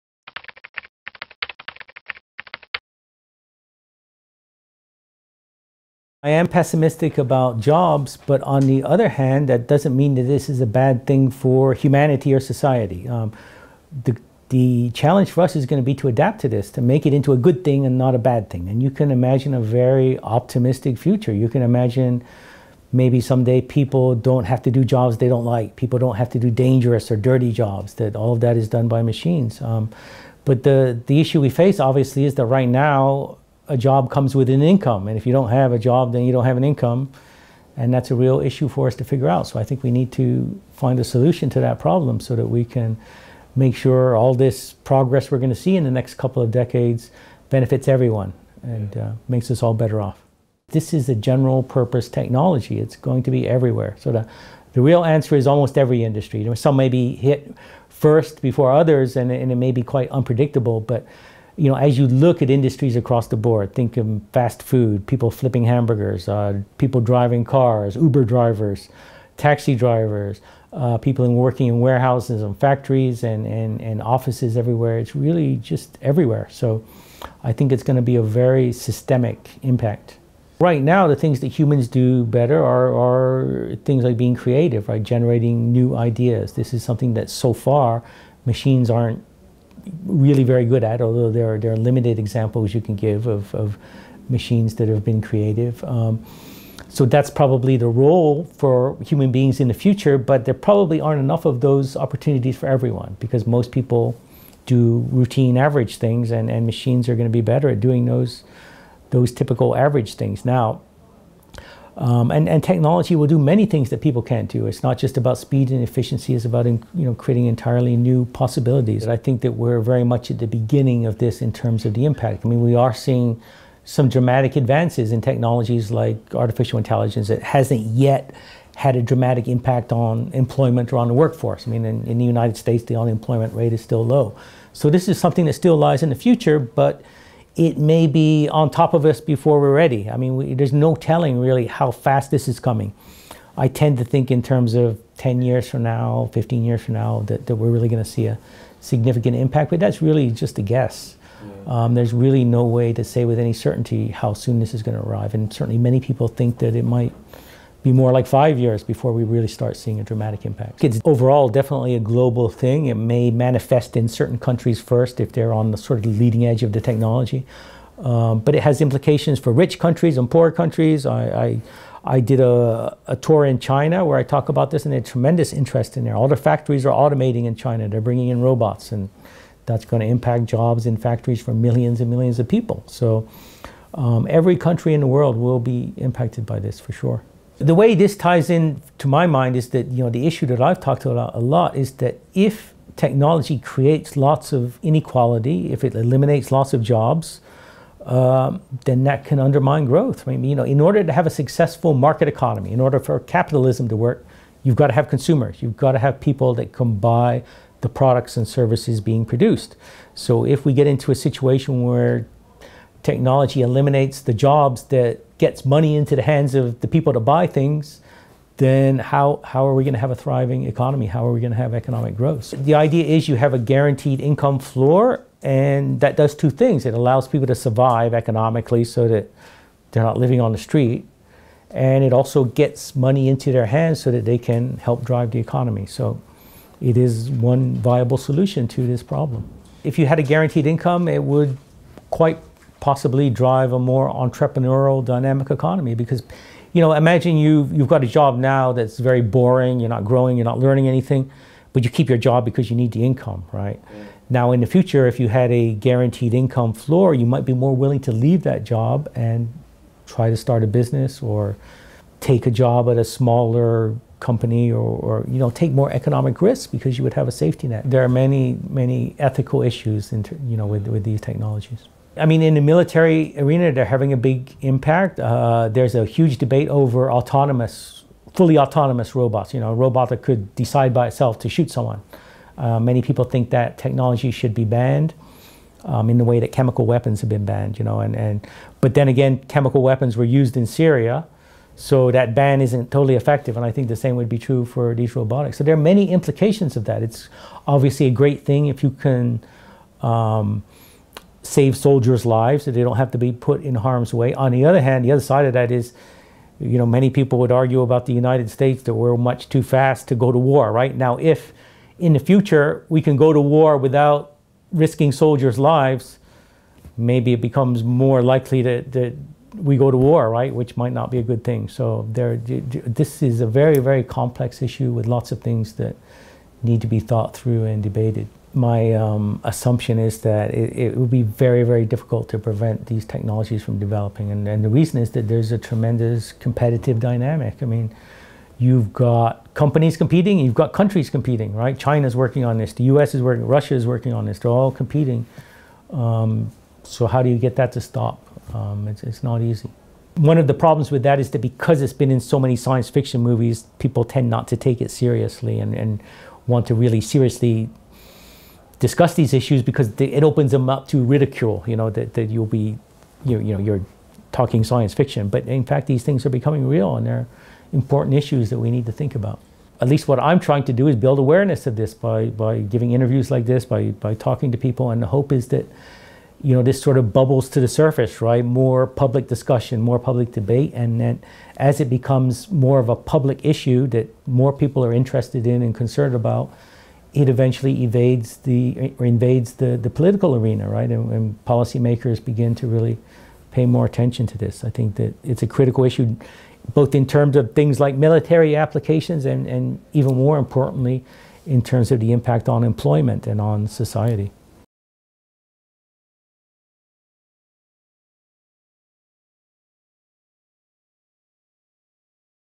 Rise of the robots: An interview with Martin Ford.
Martin Ford on technology and the threat of a jobless future. A video interview conducted at the 46 St. Gallen Symposium.

Der Aufstieg der Roboter: Ein Interview mit Martin Ford.
Autor Martin Ford über rasante technologische Entwicklungen. Werden Roboter Arbeit überflüssig machen und damit gleichzeitig viele Arbeitsplätze gefährden? Ein Videointerview anlässlich des 46. St. Gallen Symposiums. (in Englisch)